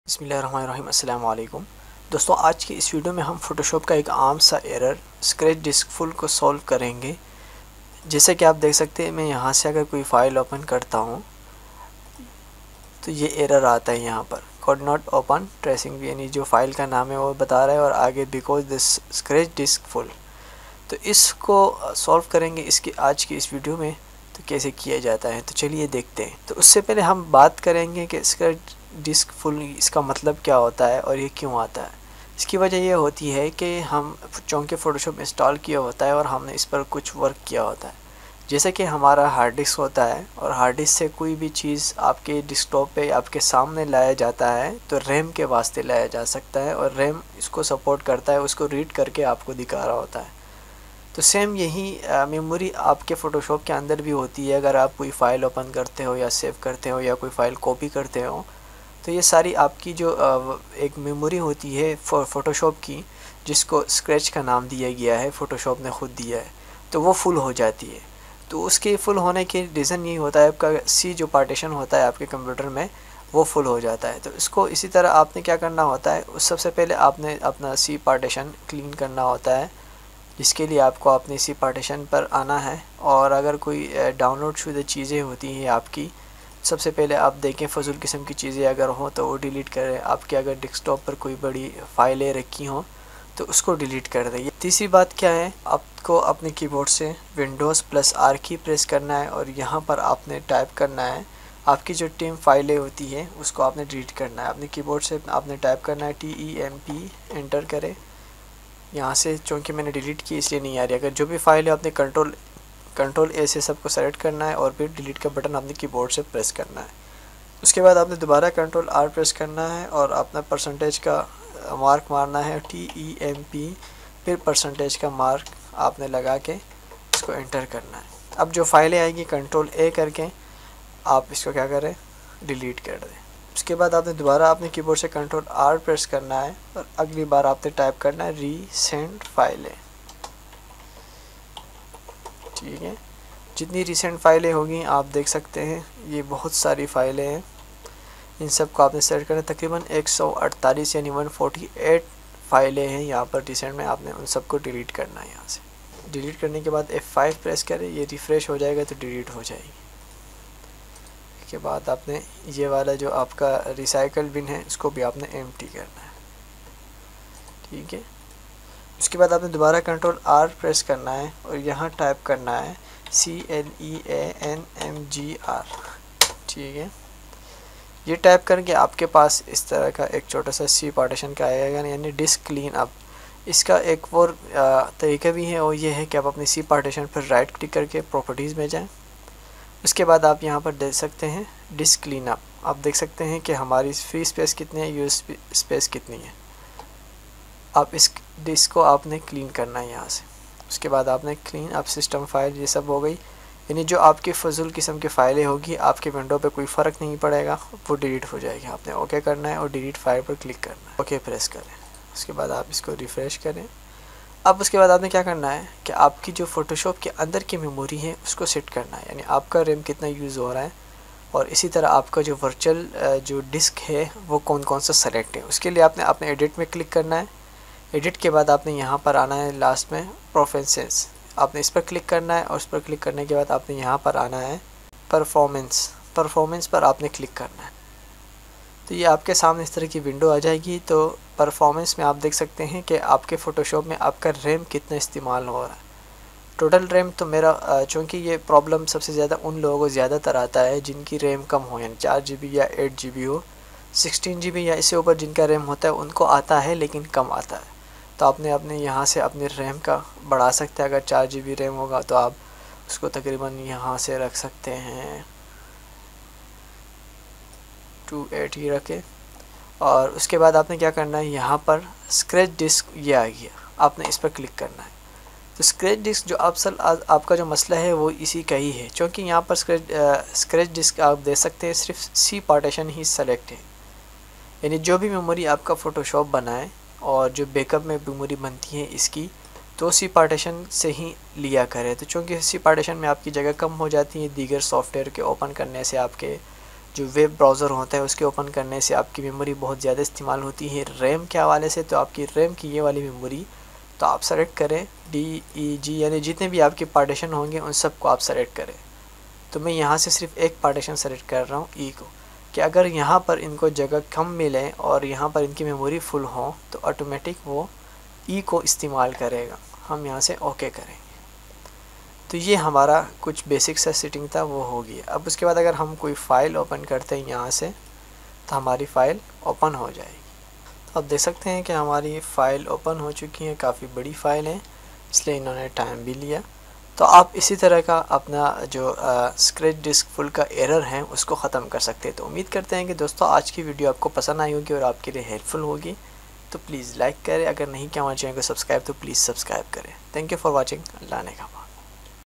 100 % Solved Scratch Disk Full Error In Photoshop CC 2020 | Photoshop Memory Error | Could not open
In this video we have fixed a common problem "Scratch Disk Full" in Photoshop.
Could not open Because the scratch disks are full solved

save your time & money on online Learning